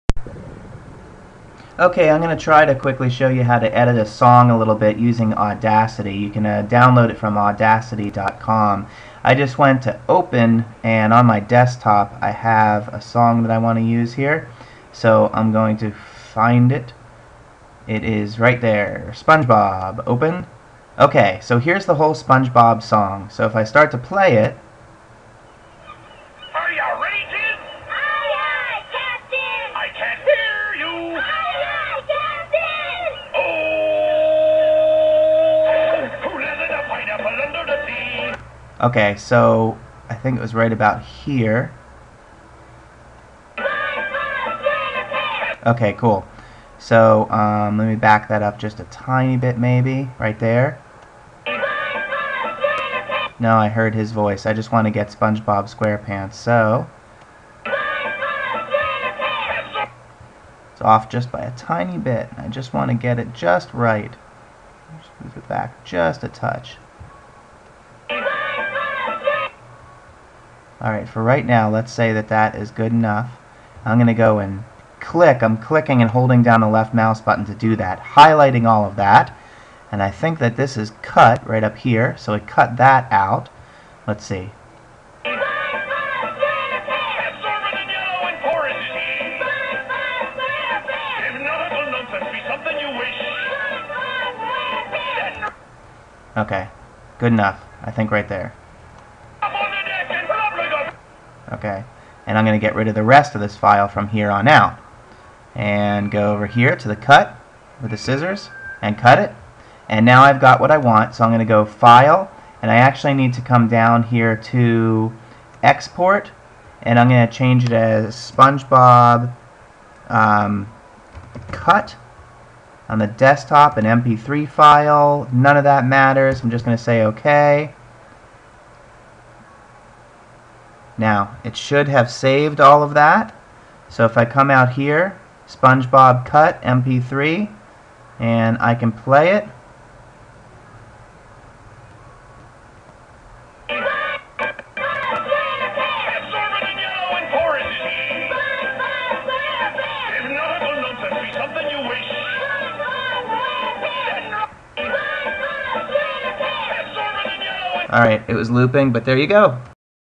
audacity to cut a song
Using audacity to cut a segment of a song.  We claim no responsibility or credit for this program.  We just like it.